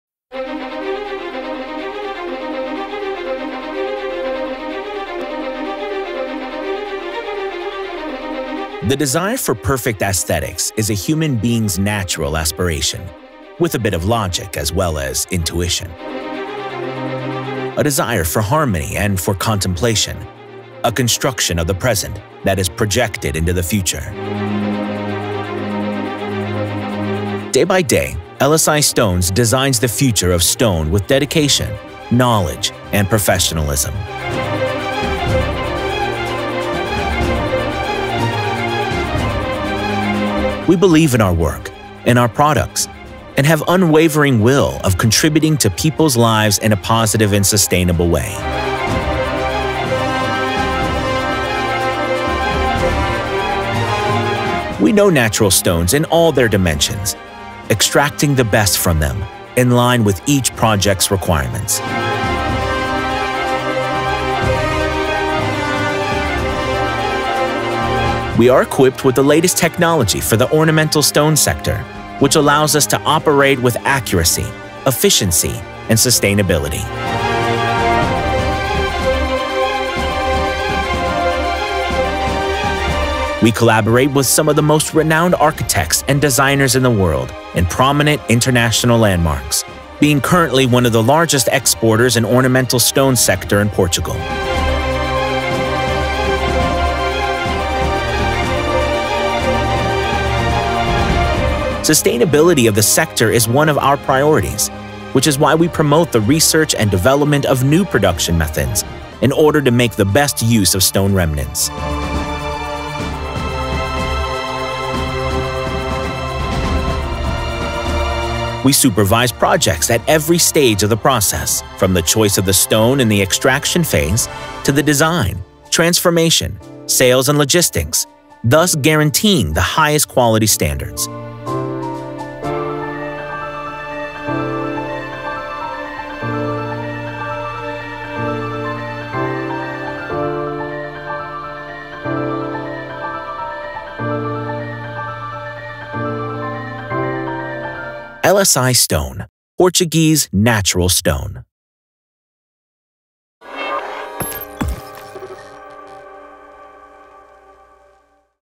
LSI Stone | Our Factory
LSI Stone is a Portuguese company specialized in the supply and processing of natural stones. It is located in the heart of Estremadura’s Limestone Massif, the most important center of limestone extraction in Portugal. Limestone supplier of Moca Cream, Moleanos, Ataija, and more.